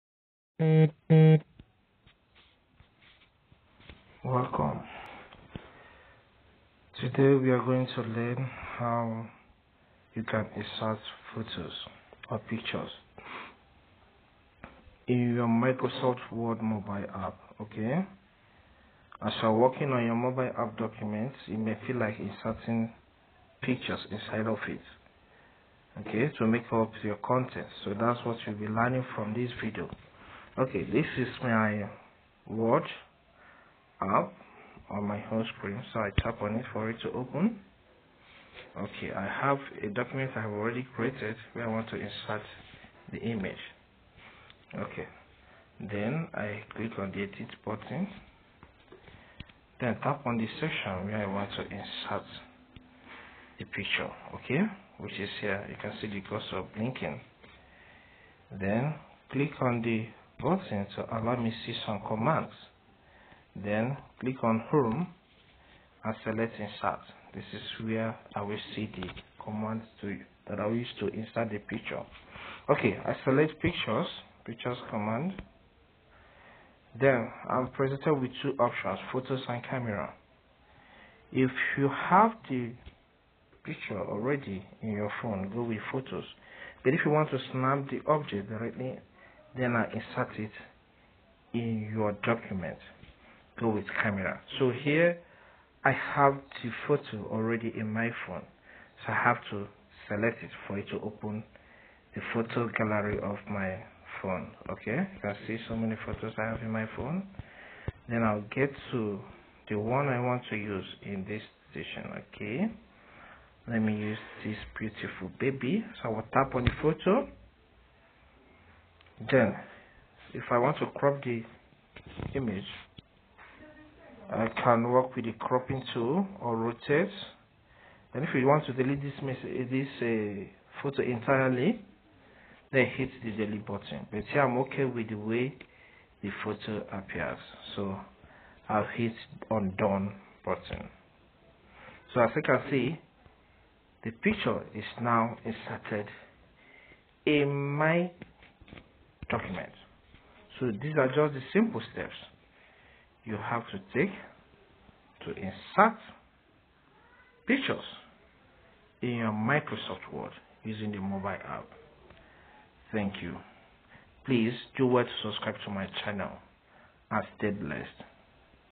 How to Insert Pictures in Microsoft Word Mobile App - Easily Insert Images in Word Mobile
This short video will guide you on how to insert pictures in Microsoft Word mobile app. If you work with MS Word mobile app, this video will guide you on how to insert images in it. The mobile version of the Word editor allows users to work on the go. This is because it is handy.
To insert image in document, tap the ribbon display option followed by Home to access the other tabs of the program. Select Insert and Picture command. You can tap on the image you want to use and it will be placed in the document.